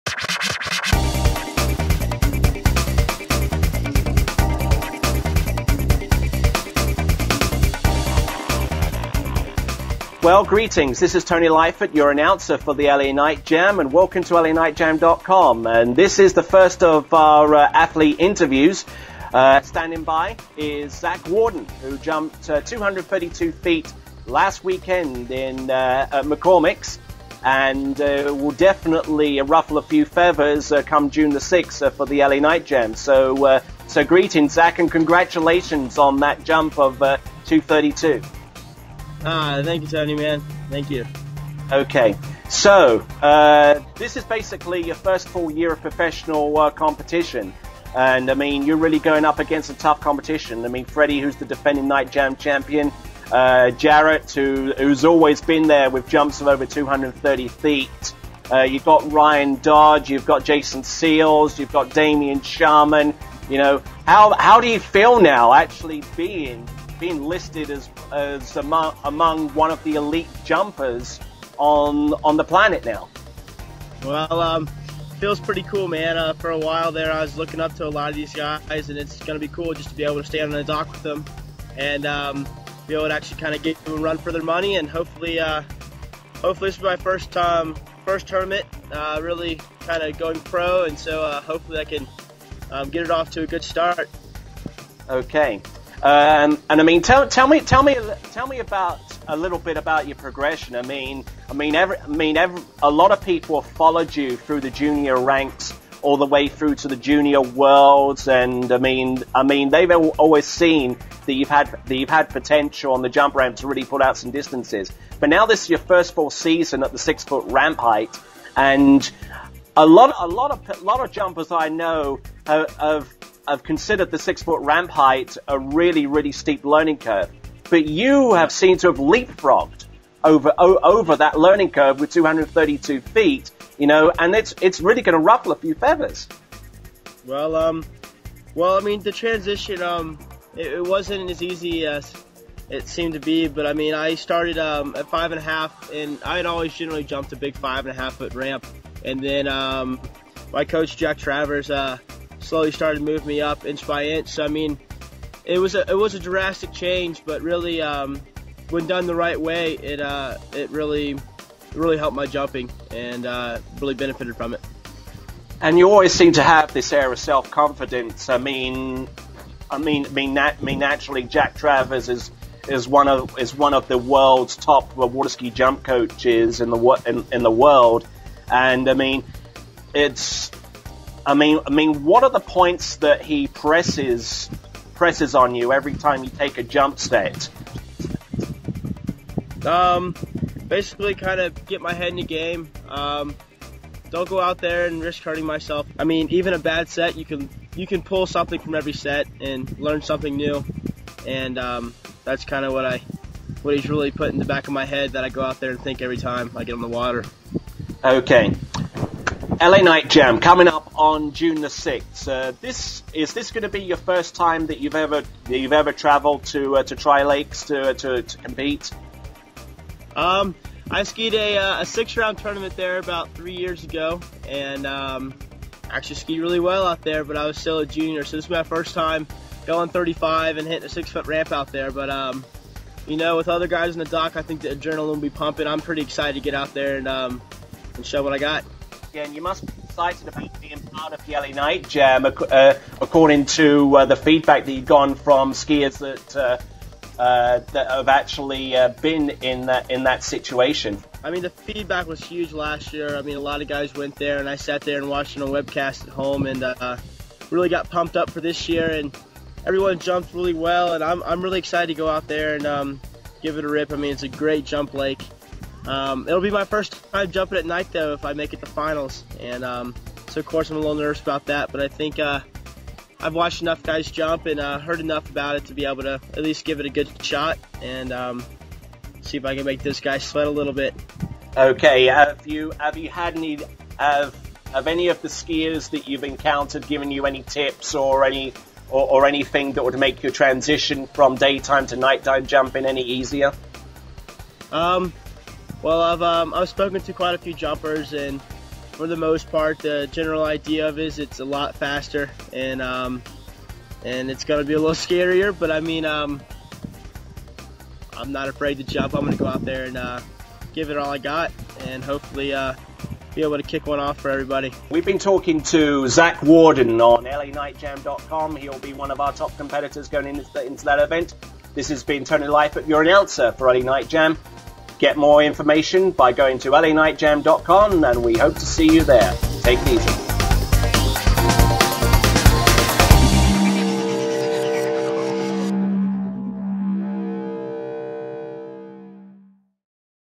Zack Worden - Audio Interview
LA Night Jam Announcer Tony Lightfoot talks with Zack Worden about his performances so far this season and his participation in the forthcoming LA Night Jam.